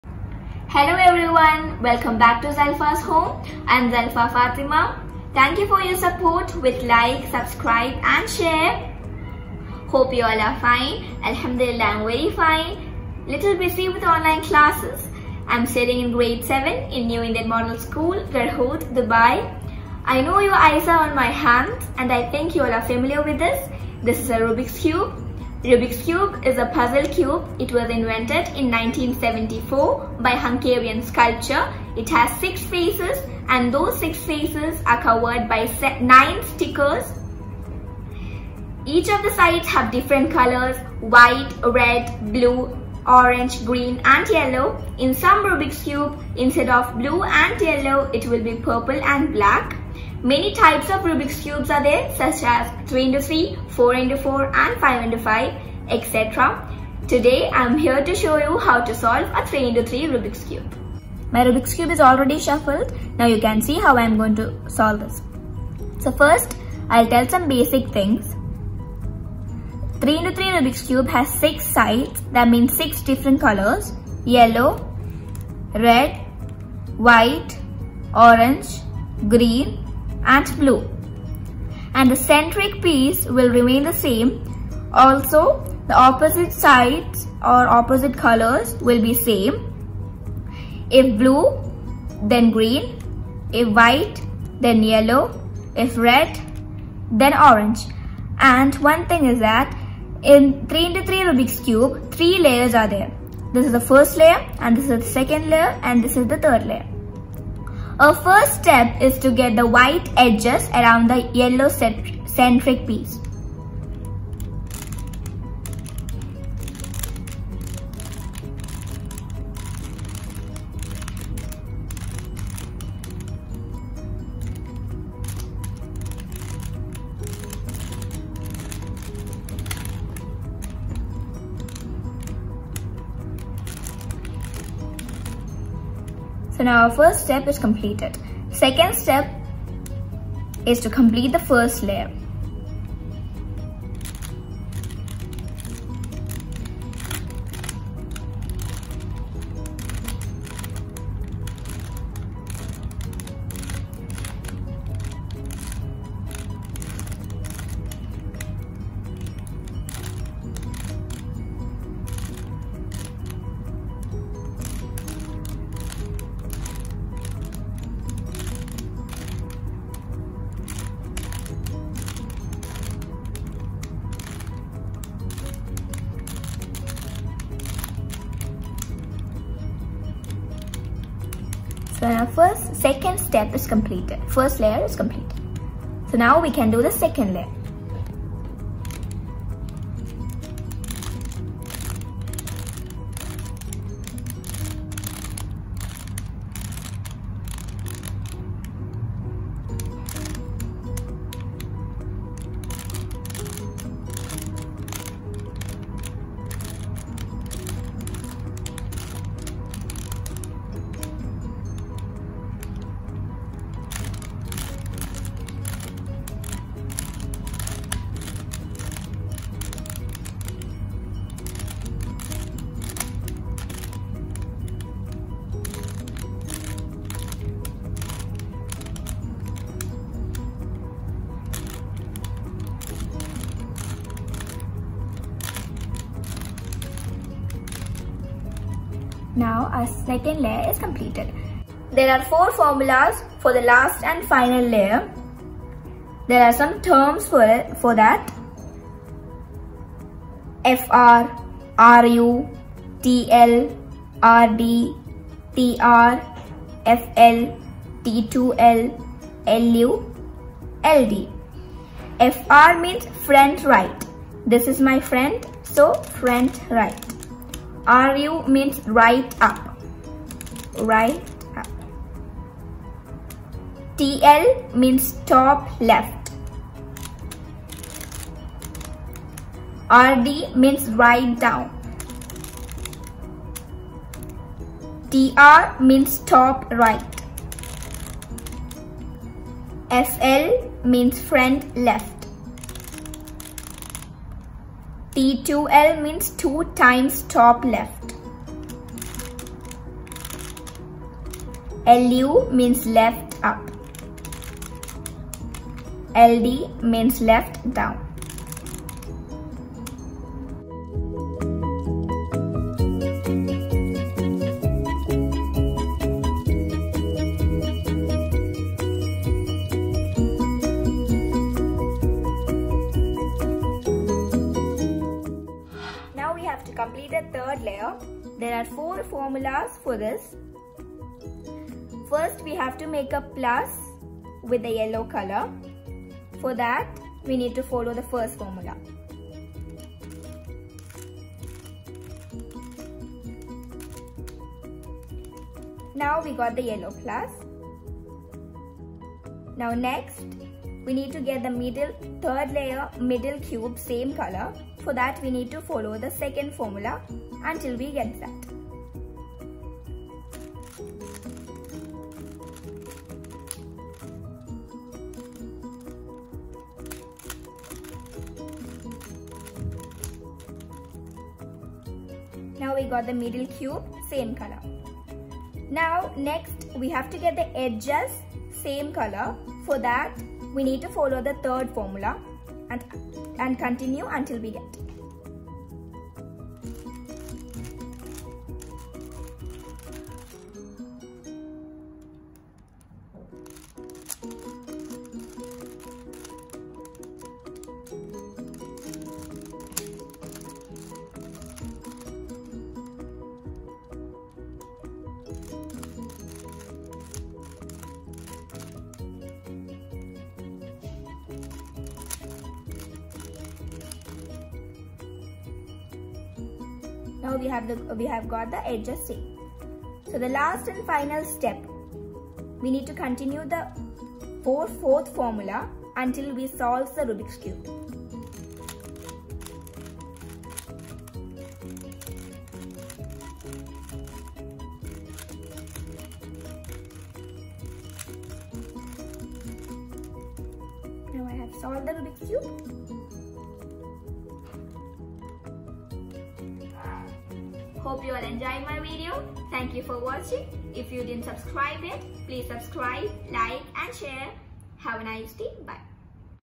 How to Solve Rubik’s Cube 3x3, Zalfa’s Home.
Rubik's Cube is a 3-D combination puzzle invented in 1974 by Hungarian sculptor and professor of architecture Ernő Rubik. Originally called the Magic Cube, the puzzle was licensed by Rubik to be sold by Ideal Toy Corp. in 1980 via businessman Tibor Laczi and Seven Towns founder Tom Kremer.
There are different types of Rubik's Cube. 3x3, 4x4, 5x5 etc. Here I am showing in this video, how to solve , Rubik's Cube 3x3.

Zalfa's Home.